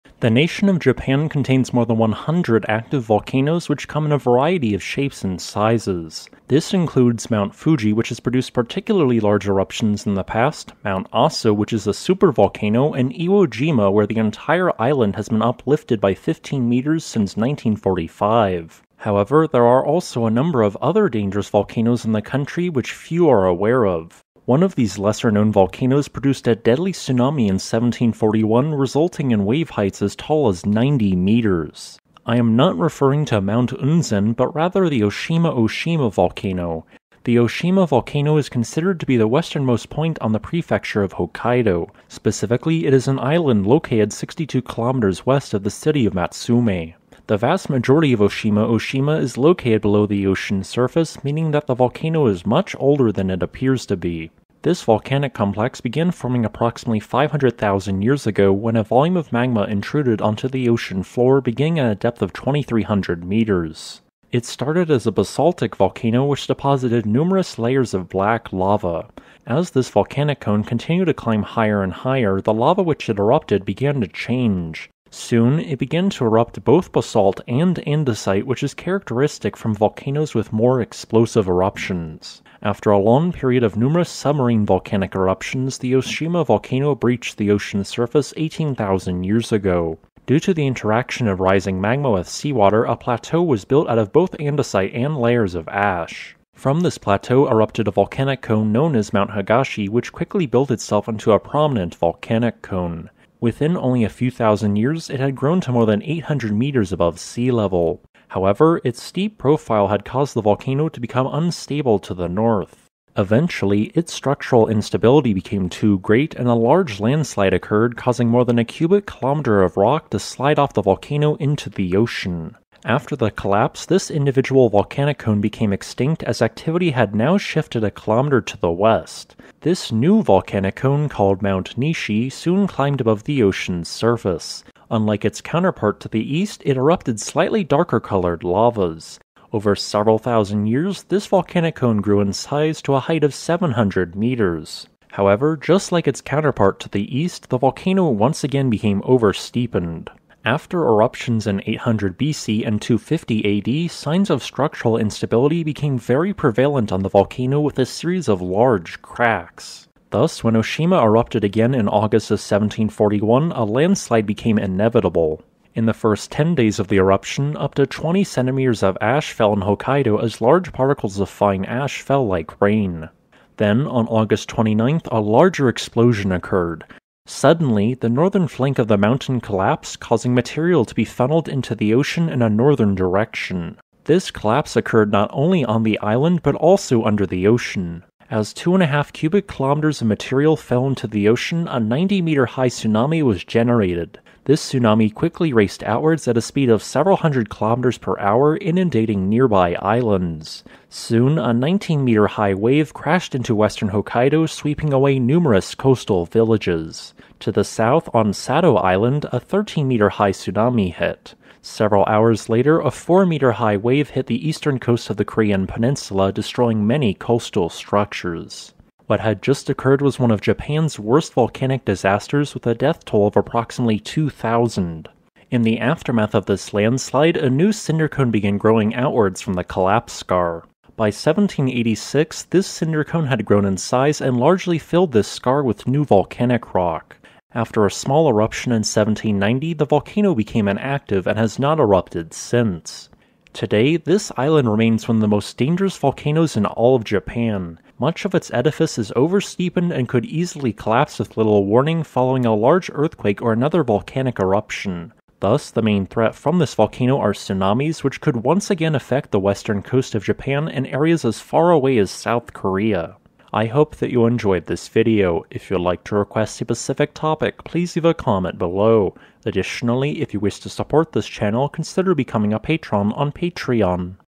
The Active Volcano in Japan; Oshima-Oshima
The nation of Japan contains more than 100 active volcanoes which come in a variety of shapes in sizes. This includes Mount Fuji, which has produced particularly large eruption in the past, Mount Aso which is a supervolcano, and Iwo Jima, where the entire island has been uplifted by 15 meters since 1945. However, there are also a number of other dangerous volcanoes in the country which few are aware of. One of these lesser known volcanoes produced a deadly tsunami in 1741, resulting in wave heights as tall as 90 meters. I am not referring to Mount Unzen, but rather the Oshima Oshima volcano west of mainland Hokkaido. This video will discuss this volcano and mention what its future hazards are.

0:00 Japan's Numerous Volcanoes
0:24 Oshima Oshima Volcano
0:37 Location of Oshima Oshima
0:55 Geologic History
2:40 1741 Eruption & Collapse
3:20 Path of the Tsunami

Thumbnail Photo Credit: Yosh Ginsu yoshginsu, Unsplash, CCO 1.0